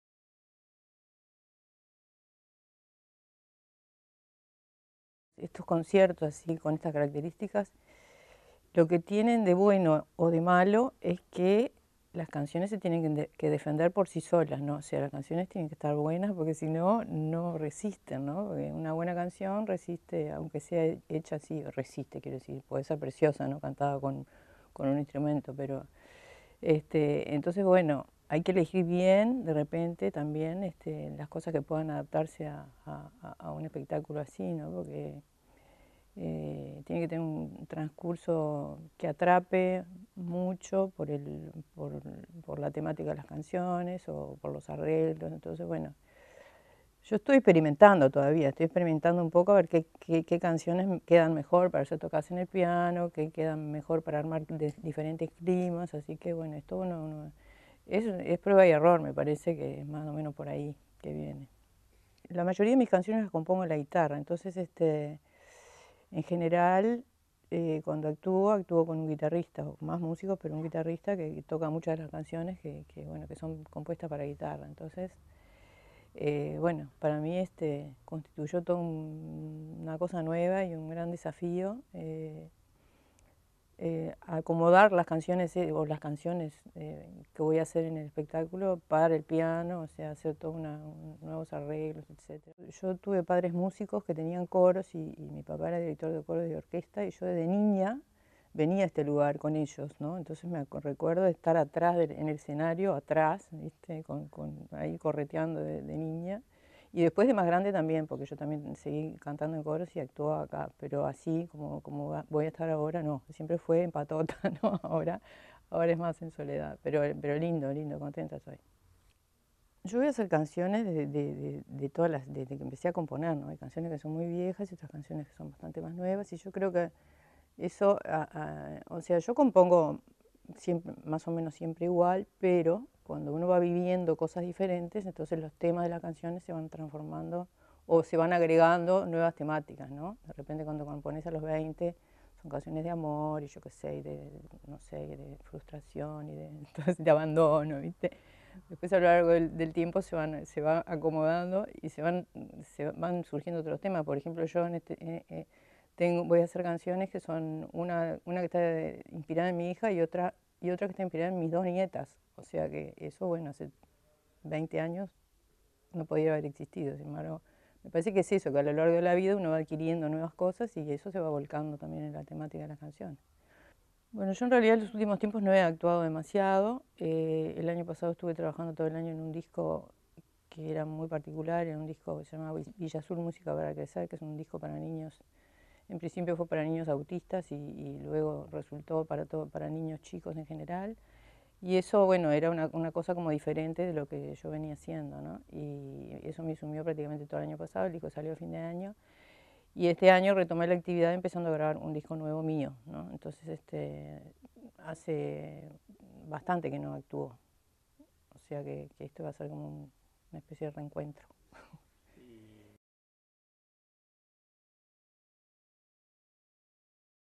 Estela Magnone en Auditorio Tv
'A lo largo de la vida uno va adquiriendo nuevas experiencias y es se va volcando en la temática de las canciones'

Estela Magnone contó cómo va a ser su concierto dentro del ciclo 'Grandes Solos'  y sus recuerdos del Auditorio.

Auditorio Nacional Sodre. Montevideo, agosto de 2015